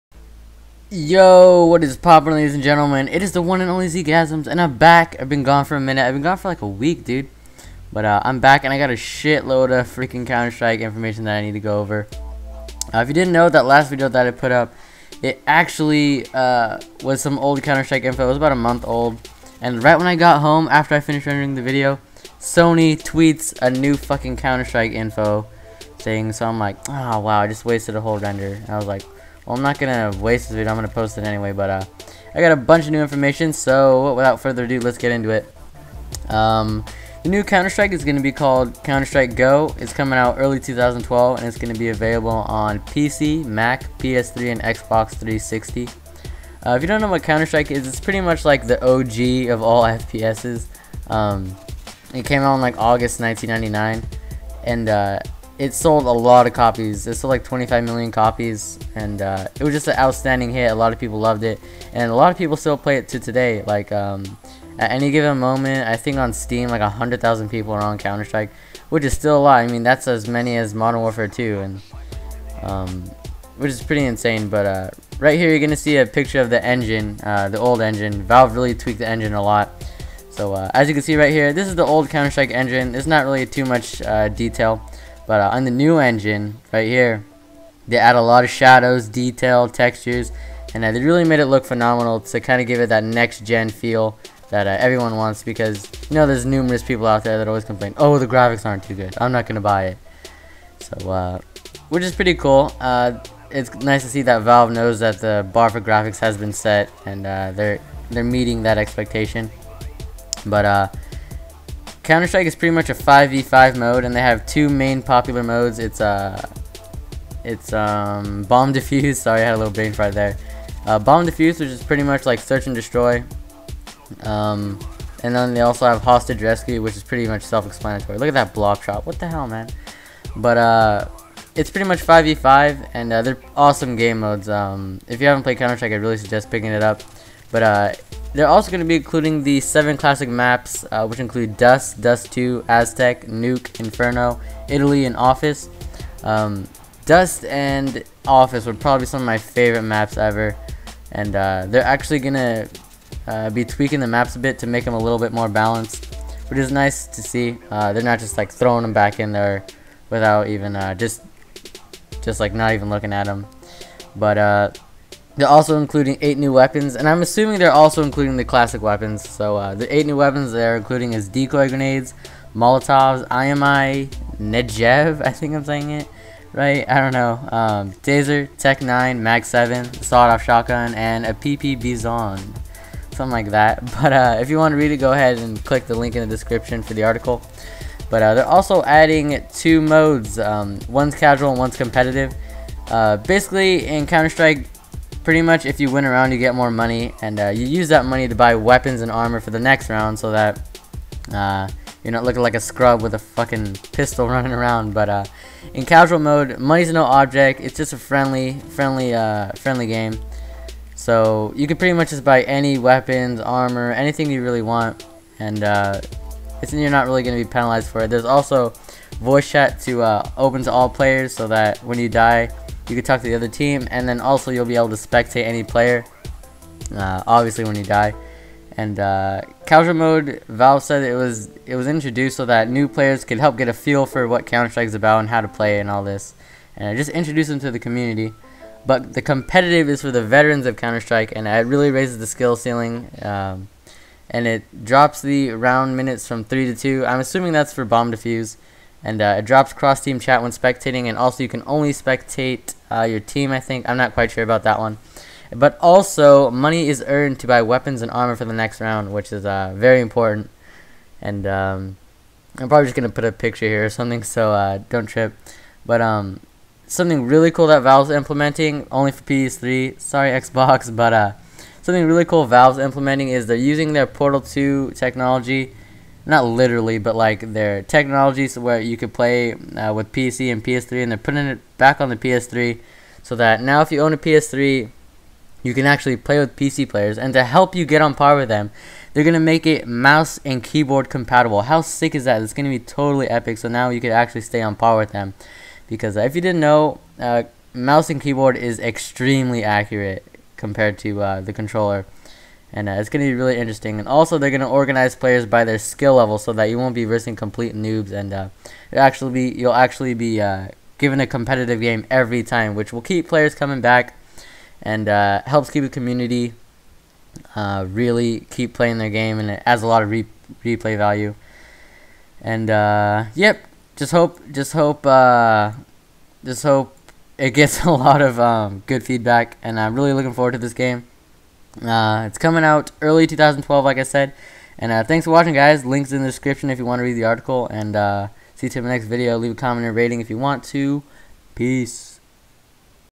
NEW COUNTER STRIKE INFO! COMMANDOG SIGHTED! PS3 vs PC capable!
Sooo, I forgot to say in the commentary, I tried to record the live squad callouts and super stardust and they didnt go well. People kept interrupting me when recording and sometimes the games were too long. I'll try to get the games in this weekend.

After Watching the trailer here is the list of guns

SMG: MP7, MP9, UMP-45, P90
Rifles: Scar 20, Famas, M4A1, SSG 08, AUG, AWP
Gear: Zues x27, Molotov, Decoy Grenade, Flashbang, High Explosive Grenade,
Smoke Grenade

That's all that I found after watching the trailer. They didn't go over the LMG's or the Pistols.